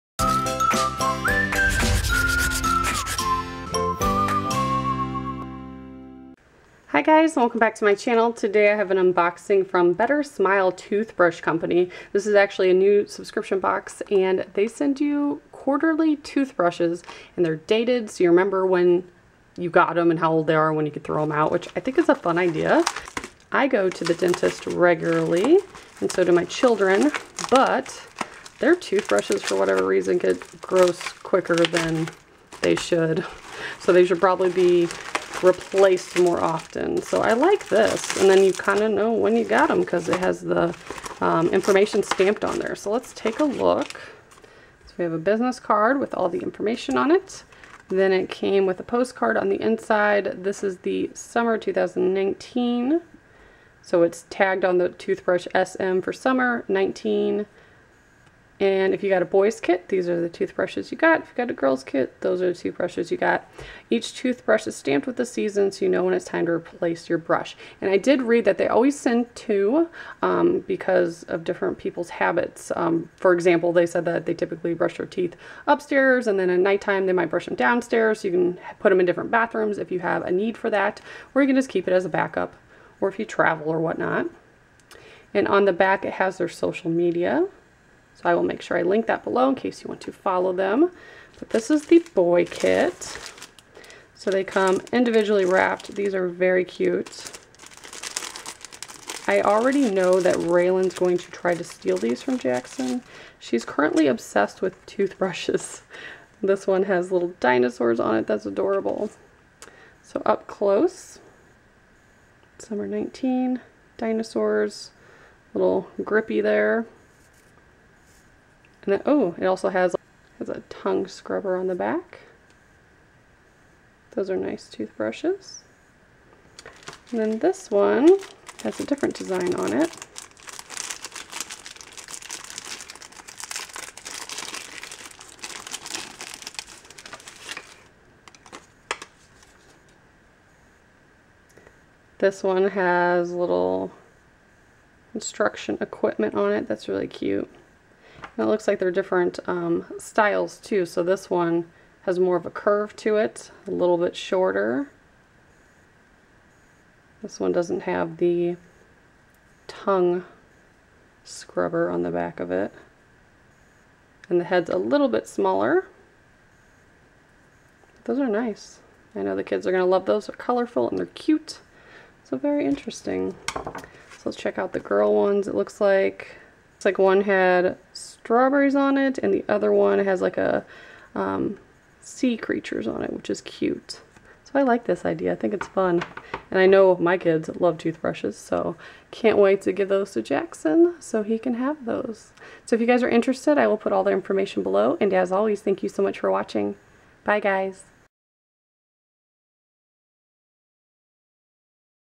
Better Smile Toothbrush Company - Kid's Subscription Unboxing!
Kid-designed toothbrush subscription
Let us give you one less thing to think about! Our toothbrushes are dated with the season and year so you know when to replace. So long gross, old toothbrushes!
• Fun and kid-designed with dates to help you de-clutter old toothbrushes

Product Overview
• Your mailer will ship on 1st of every quarter (March 1, June 1, September 1 and December 1). Please order by the end of the month to get the current mailer.
• Ships worldwide from United States
• Subscriptions automatically renew. Cancel anytime

What’s on my nails? Squeeze the day